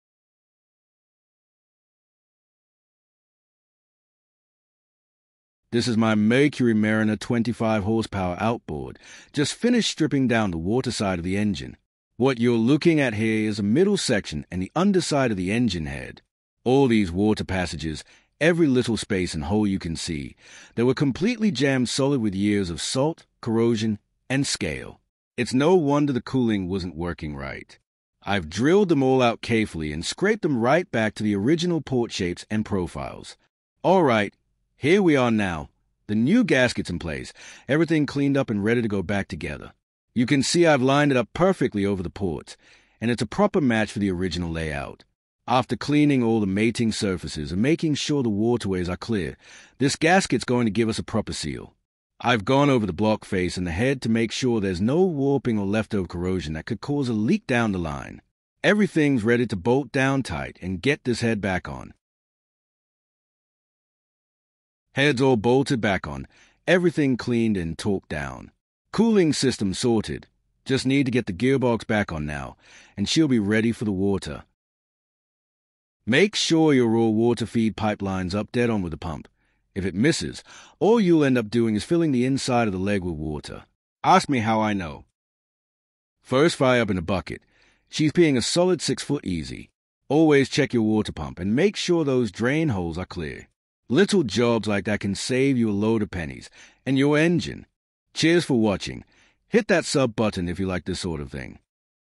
Tohatsu / Mercury Mariner 25hp Outboard – Overheating Fix & Water Pump Rebuild fire up!
In this video, I tackle a common issue with the Mercury Mariner 25hp — the water pump was severely blocked. Over time, saltwater had been trapped in the engine, crystallizing and causing corrosion in the waterways.

The main culprit? Blocked drain holes in the water pump, which prevented proper water flow, causing saltwater to stay inside the engine and do its damage. I’ve cleaned everything up, drilled out the blocked holes, and restored the water passages back to their original profiles.

I’ve also installed a brand new water pump, ensuring everything’s working properly. As for the gearbox oil? Surprisingly, it looked mint — though I didn’t film the oil change in this video.

Check out the steps I took to fix the cooling system, and how I got everything back to running smoothly. Keeping your water pump and cooling system clear is key to avoiding future issues and keeping your engine in top condition.